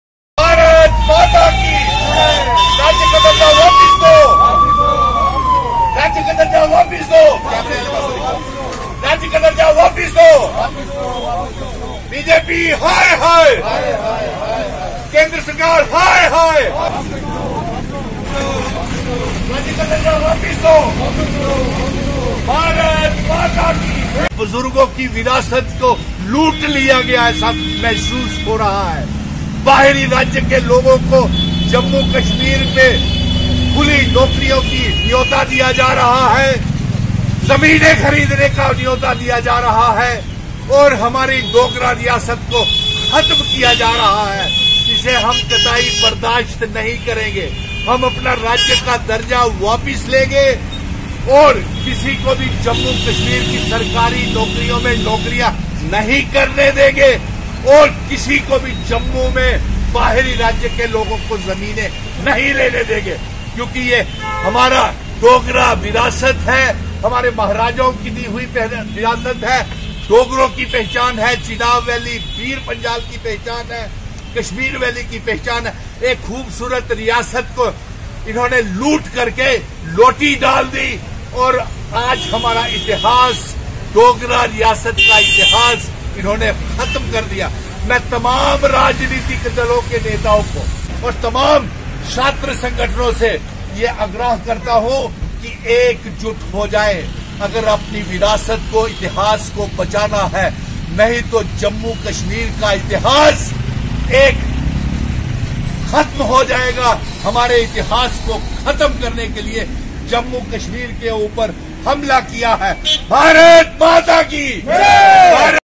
Sunil Dimple Protested against Central Government to demand restoration of statehood
Report
by

Suchet Khanna